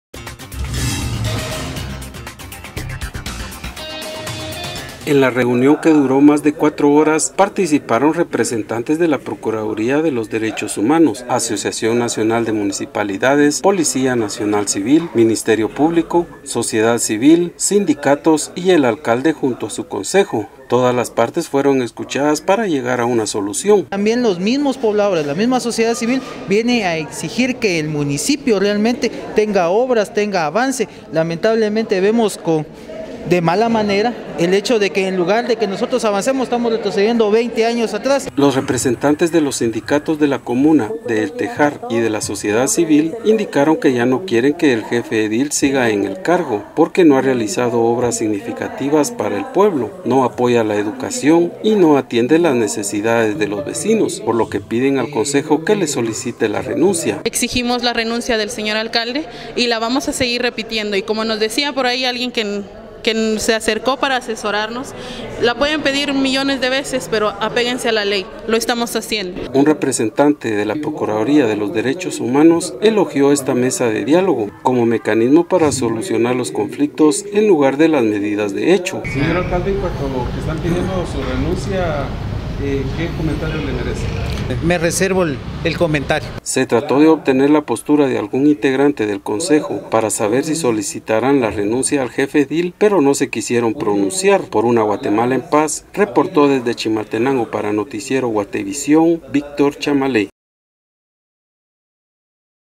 Pobladores solicitan la renuncia del alcalde de El Tejar
Los pobladores demandan la reparación de una pasarela sobre la Carretera Interamericana y rechazan la baja ejecución de obras municipales.